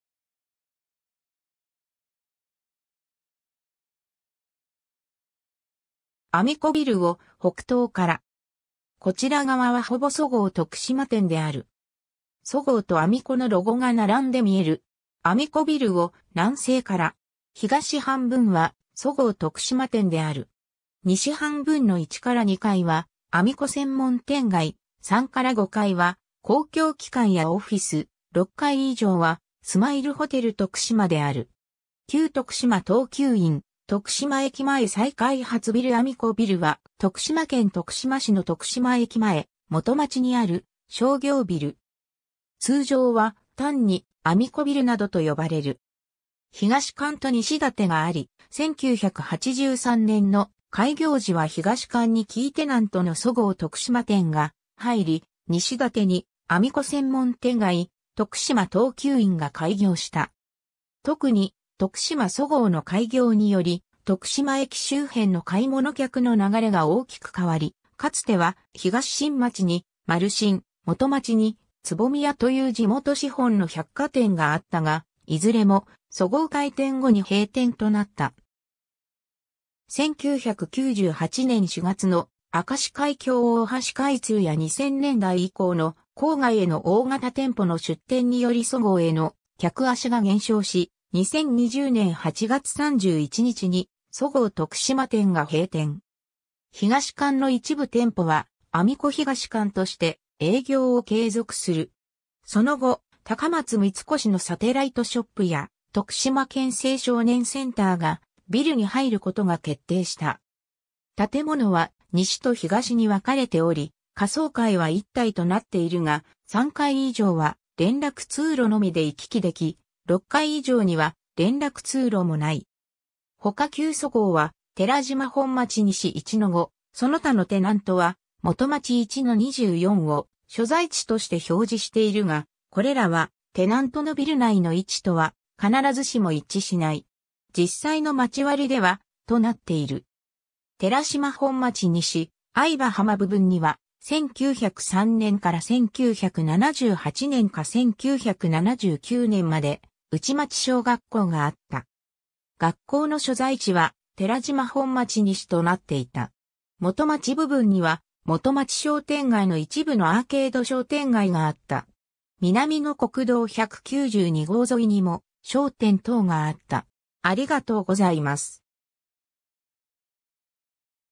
徳島駅前再開発ビルアミコビル
アミコビルを北東から。
こちら側はほぼそごう徳島店である。
そごう (SOGO) とアミコ (amico) のロゴが並んで見える。
 アミコビルを南西から。
東半分はそごう徳島店である。
西半分の1-2階はアミコ専門店街、3-5階は公共機関やオフィス、6階以上はスマイルホテル徳島である。
 旧徳島東急イン 徳島駅前再開発ビルアミコビル（とくしまえきまえさいかいはつビルアミコビル）は、徳島県徳島市の徳島駅前、元町にある商業ビル。
通常は単にアミコビルなどと呼ばれる。
 東館と西館があり、1983年の開業時は東館にキーテナントのそごう徳島店が入り、西館にアミコ専門店街、徳島東急インが開業した。
特に徳島そごうの開業により、徳島駅周辺の買い物客の流れが大きく変わり、かつては東新町に「丸新」、元町に「つぼみや」という地元資本の百貨店があったが、いずれもそごう開店後に閉店となった。
 1998年4月の明石海峡大橋開通や2000年代以降の郊外への大型店舗の出店によりそごうへの客足が減少し、2020年8月31日にそごう徳島店が閉店。
東館の一部店舗はアミコ東館として営業を継続する。
その後、高松三越のサテライトショップや徳島県青少年センターがビルに入ることが決定した。
 建物は西と東に分かれており、下層階は一体となっているが、3階以上は連絡通路のみで行き来でき、6階以上には連絡通路もない。
 ほか 旧そごうは寺島本町西1-5、その他のテナントは元町1-24を所在地として表示しているが、これらはテナントのビル内の位置とは必ずしも一致しない。
 実際の町割では、 となっている。
 寺島本町西・藍場浜部分には、1903年から1978年か1979年まで内町小学校があった。
学校の所在地は寺島本町西となっていた。
 元町部分には、元町商店街の一部のアーケード商店街があった。
南の国道192号沿いにも商店等があった。